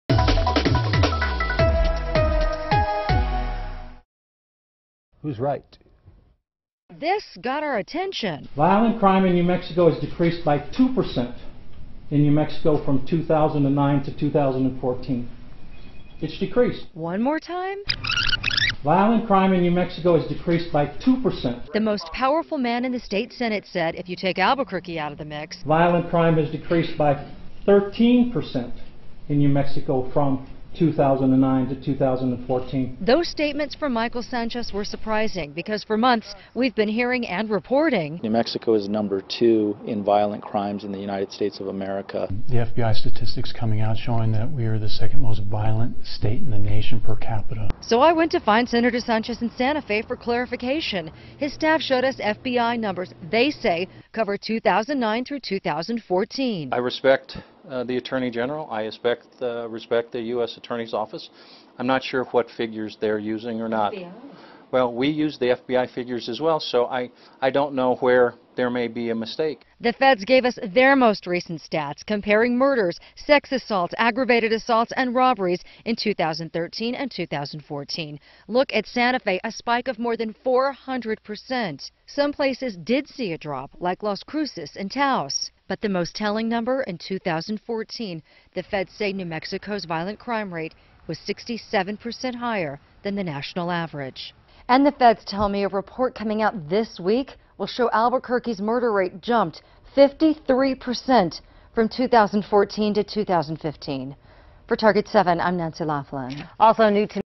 Where violent crime has increased in New Mexico
Violent crime has increased in several New Mexico cities from 2013 to 2014.